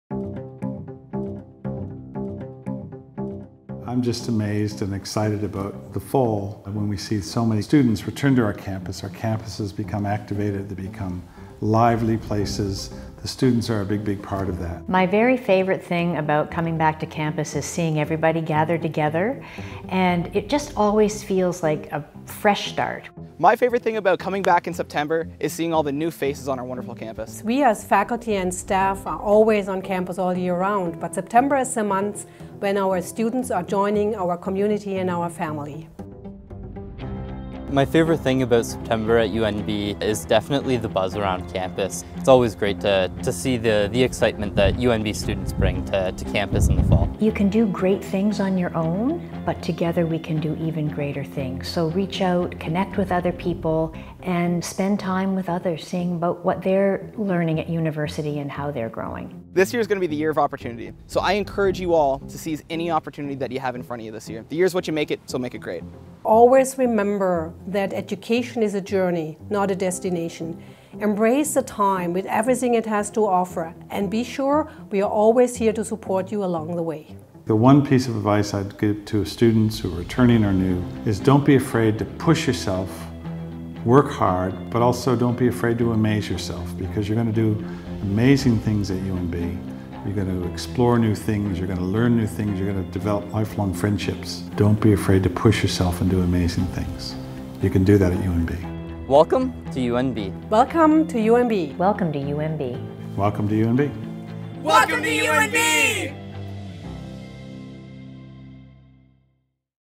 Welcome Back to Campus
September is an exciting time at UNB. New and returning students fill our campuses with an energy and enthusiasm that can be felt all around us.

Some of our student and academic leaders have come together in a video to launch the start of the new academic year. I look forward to seeing what this year will hold for all of us.